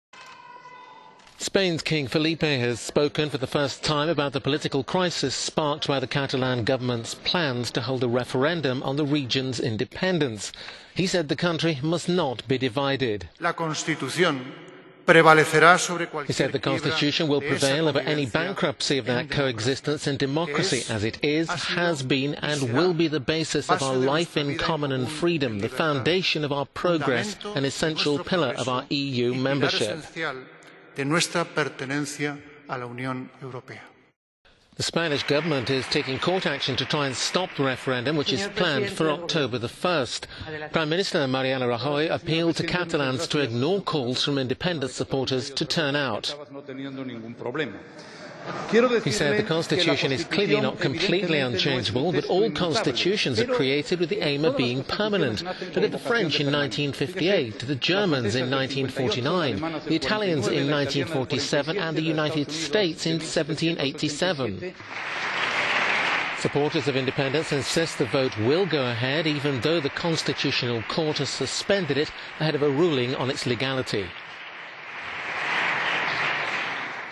King Felipe speaks out on Catalan poll plans
Spain's King Felipe VI has spoken for the first time about the political crisis sparked by the Catalan government’s plans to hold a referendum on the region’s independence.

He said the country must not be divided, adding; "The Constitution will prevail over any bankruptcy of that coexistence in democracy, as it is, has been and will be the basis of our life in common and freedom, the foundation of our progress and essential pillar of our EU membership."